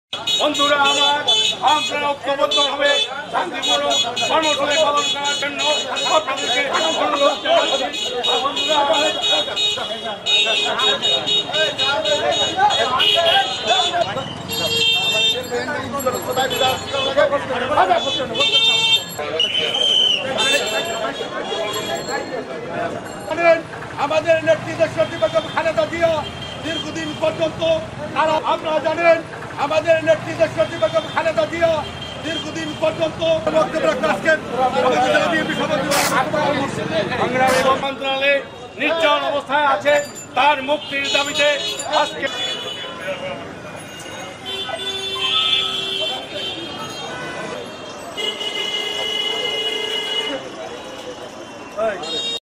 ঝালকাঠিতে বিএনপির অবস্থান কর্মসূচিতে পুলিশের বাধা- Poriborton
ঝালকাঠিতে বিএনপির অবস্থান কর্মসূচিতে পুলিশের বাধা

Poriborton Official Address:
Poriborton